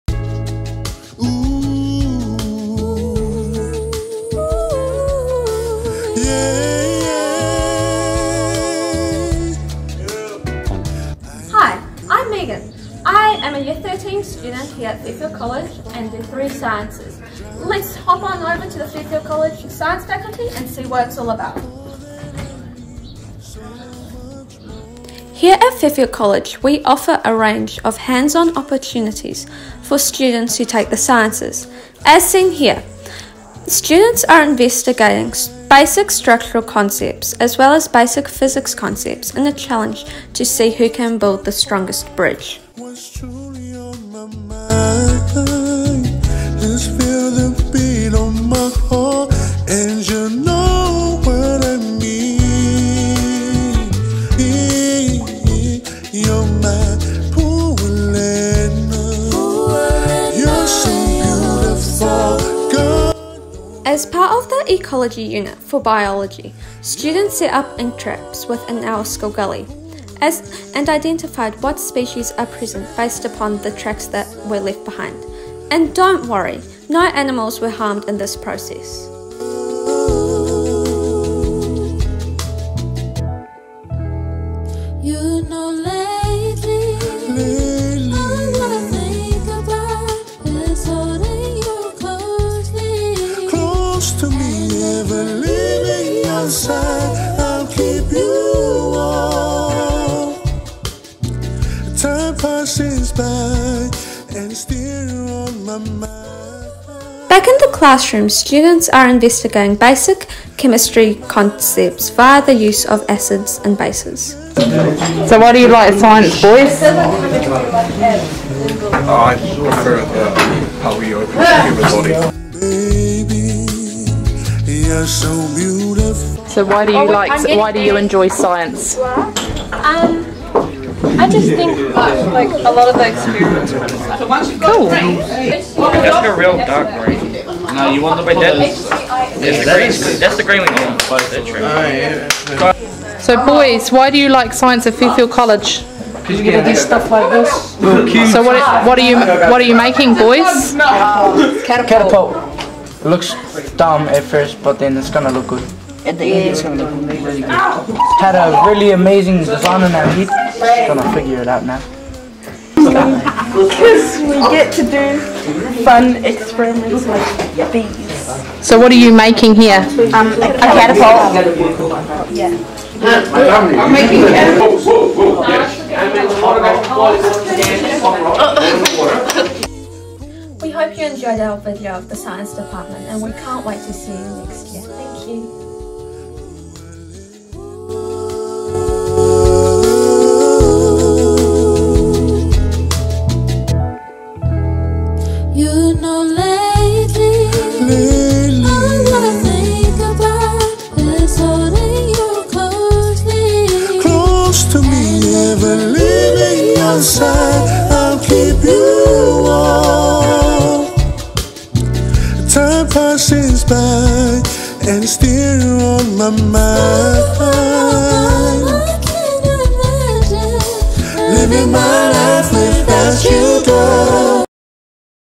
Fairfield College Science Department
Welcome to Fairfield College.  Take a tour with Year 13 student Megan Koschany as she showcases the Science Department at Fairfield College.